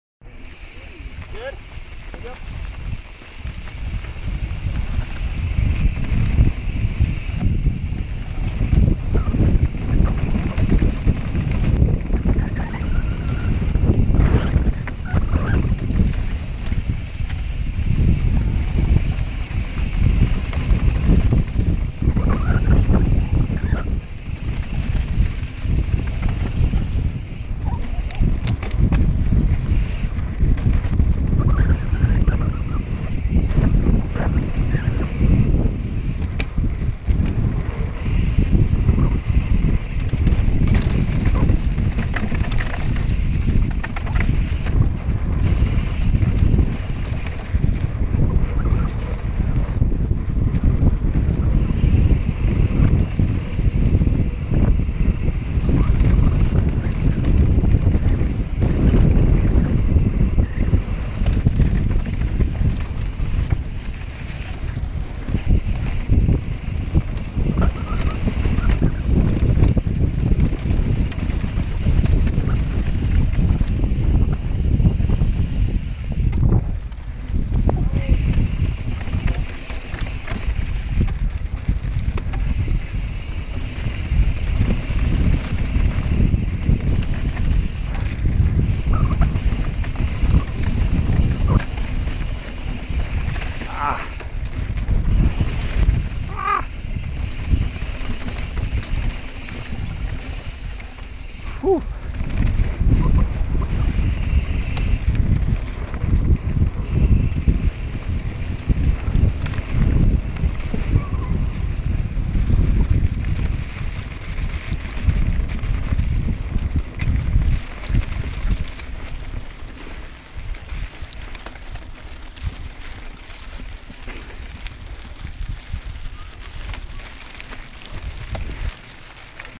Troupe Headed Down Cactus in the O.C.
A little footage from an O.C. ride that took us through Whiting Ranch and down through Cactus. The chest cam got off kilter and was basically pointing at my feet so we're left with some top o' the helmet footage as backup.